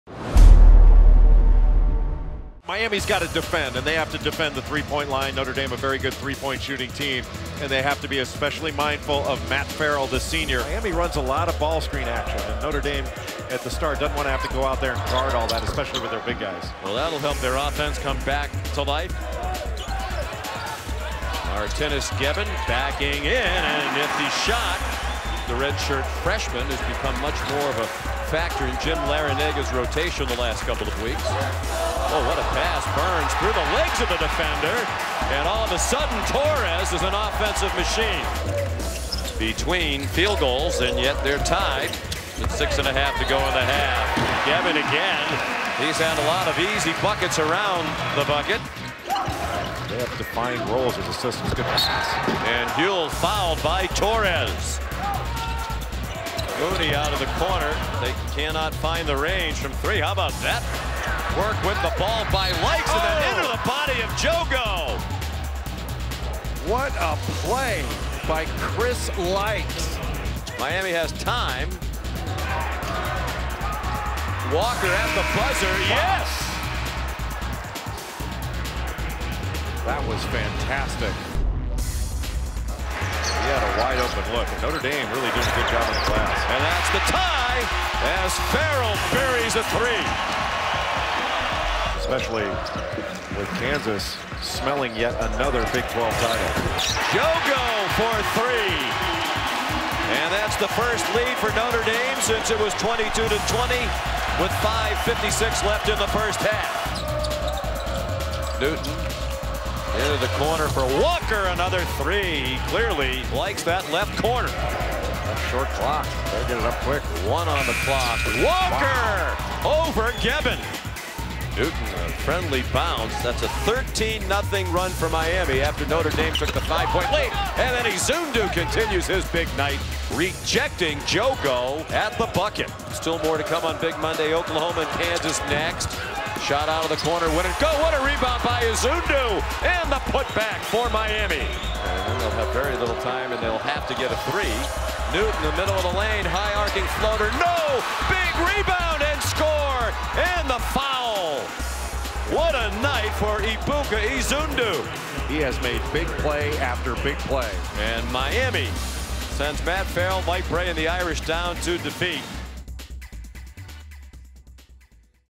Miami vs. Notre Dame Basketball Highlights (2017-18)
Miami vs. Notre Dame: Hurricanes forward Ebuka Izundu came up clutch down the stretch, scoring 12 points of his 14 points in the final 9:10 of the game as Miami got a big road win, 77-74 over the Irish. Lonnie Walker IV led the 'Canes with 19 points in a win that ended a 3-game losing streak. Martinas Geben led Notre Dame with 19 points in the loss.